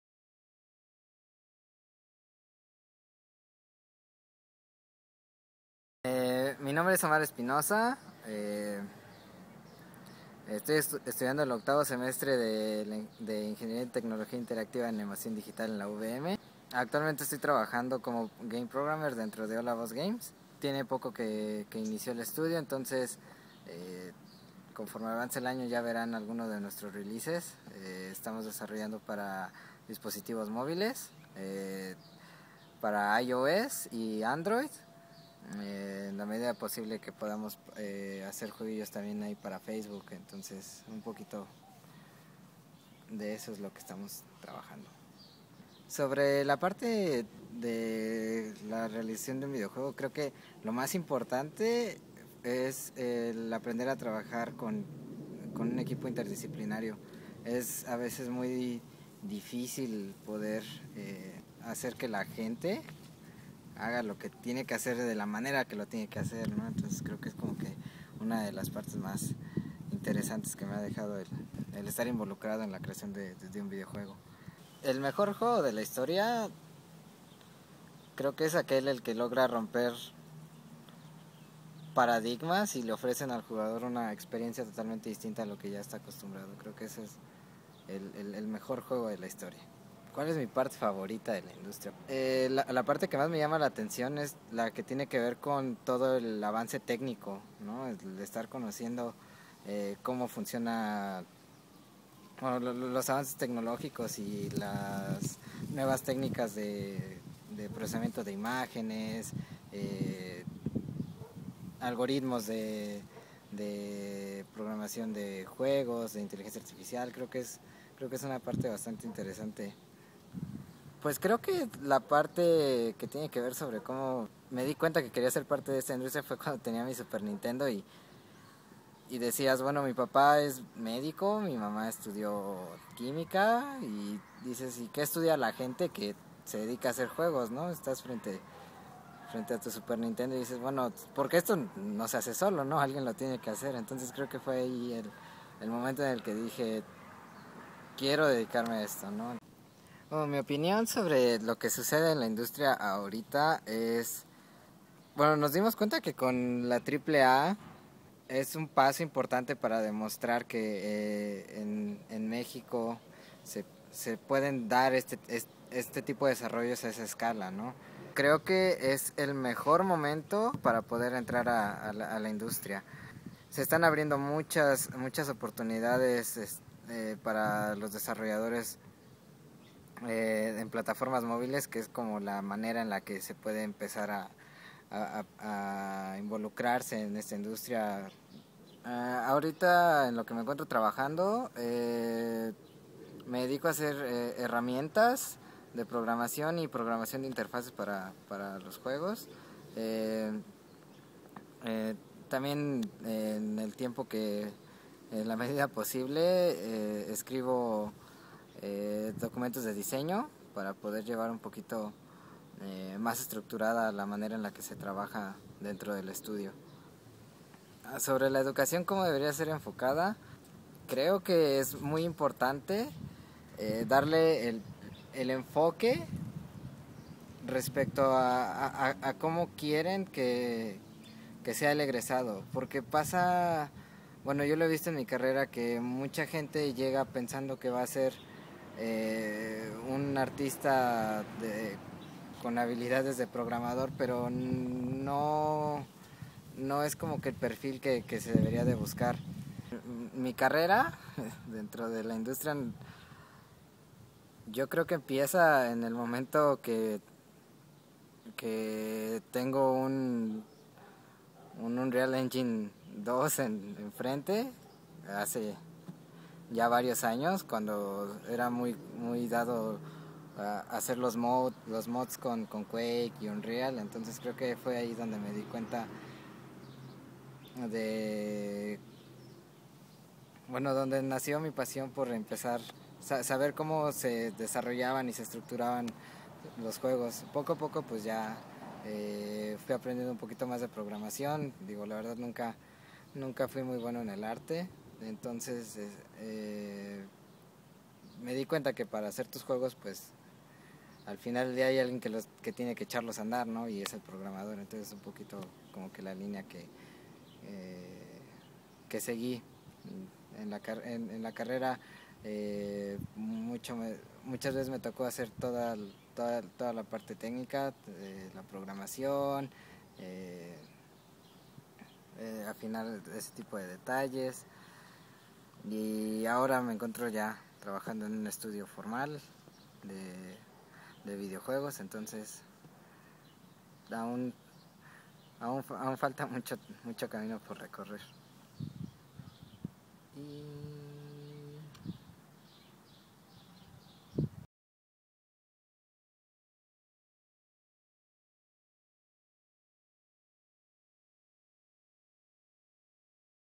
DevHour2011 Entrevista Omar Espinosa
Foro Mexicano de Profesionales del VideoJuego

Entrevista a la industria del videojuego con Omar Espinosa.